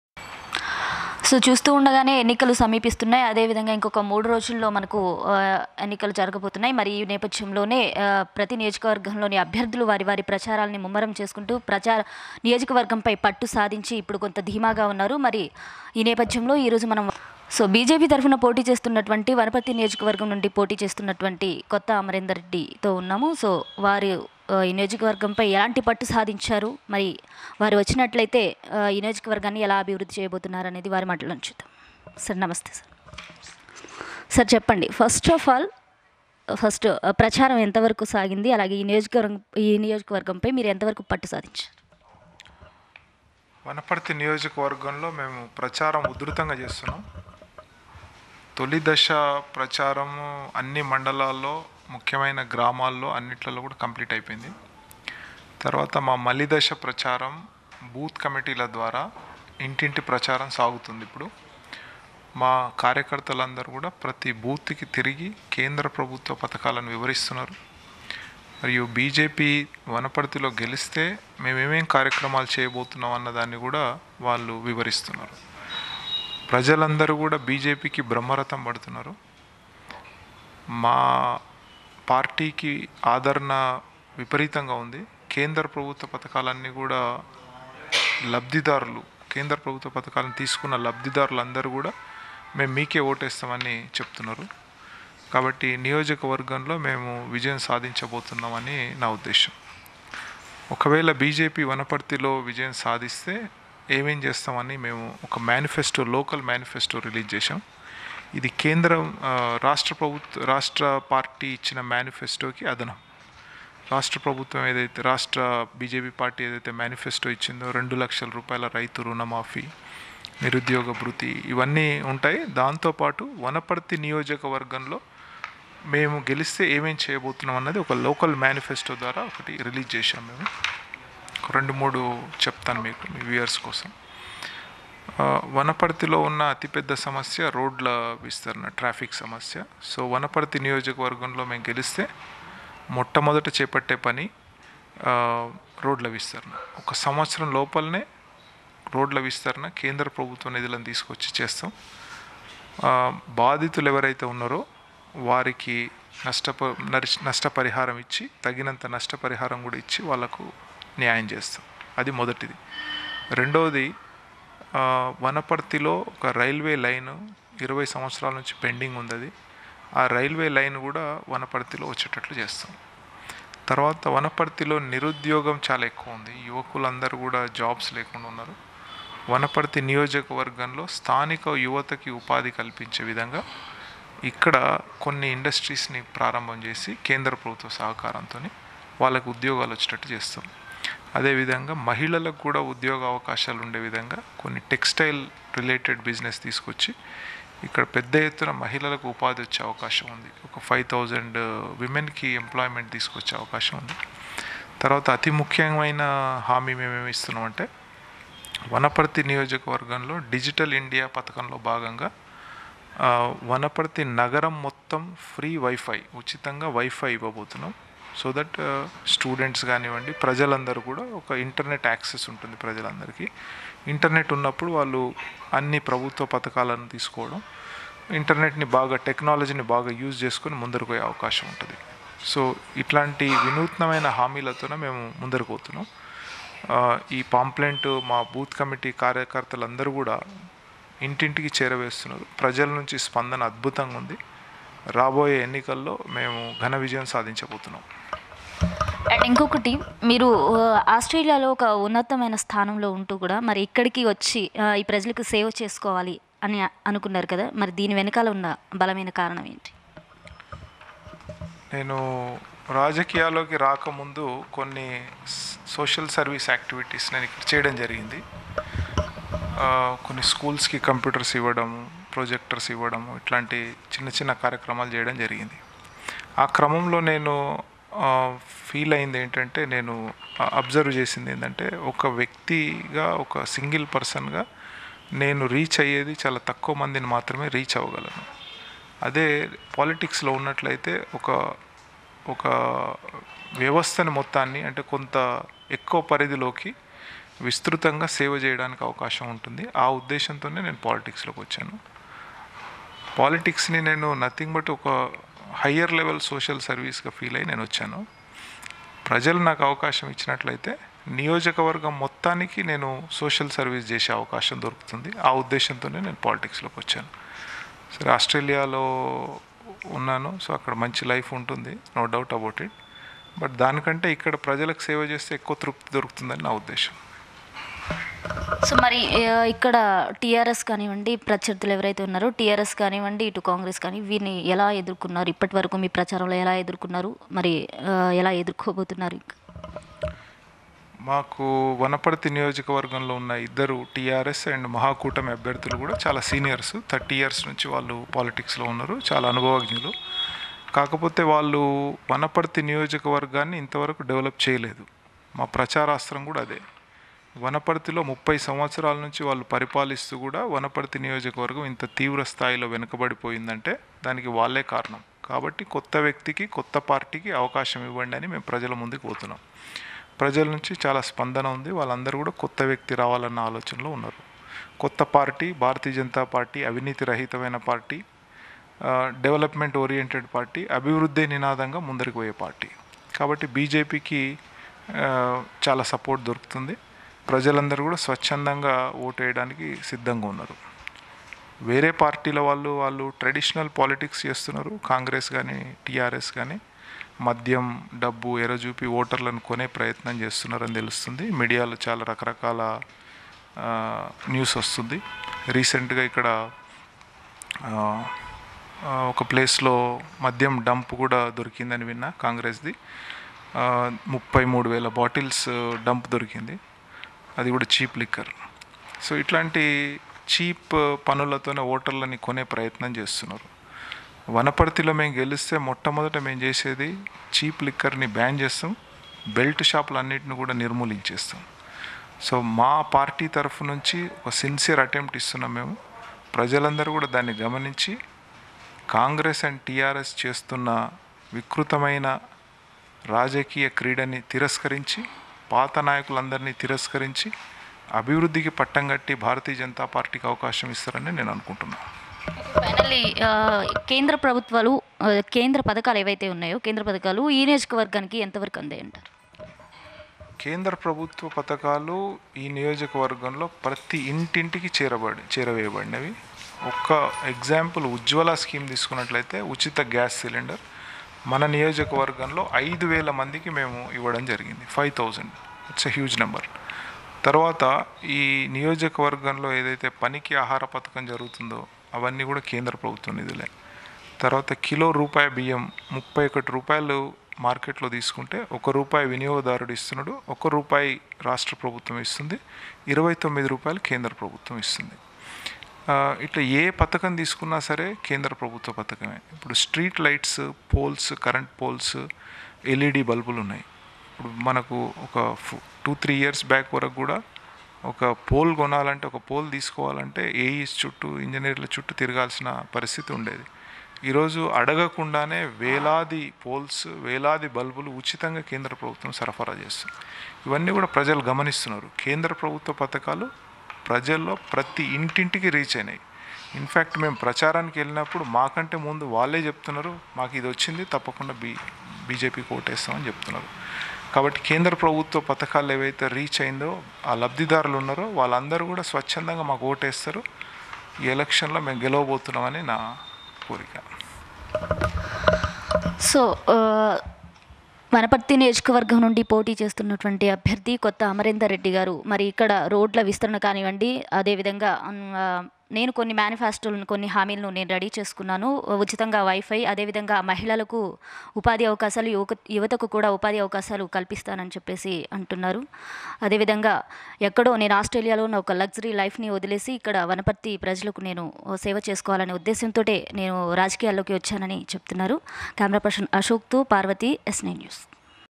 S9 NEWS MBNR వనపర్తి బీజేపీ అభ్యర్థి కొత్త అమరేందర్ రెడ్డి తో ముఖాముఖి
MAHABUBNAGAR -OUR DISTRICT OUR NEWS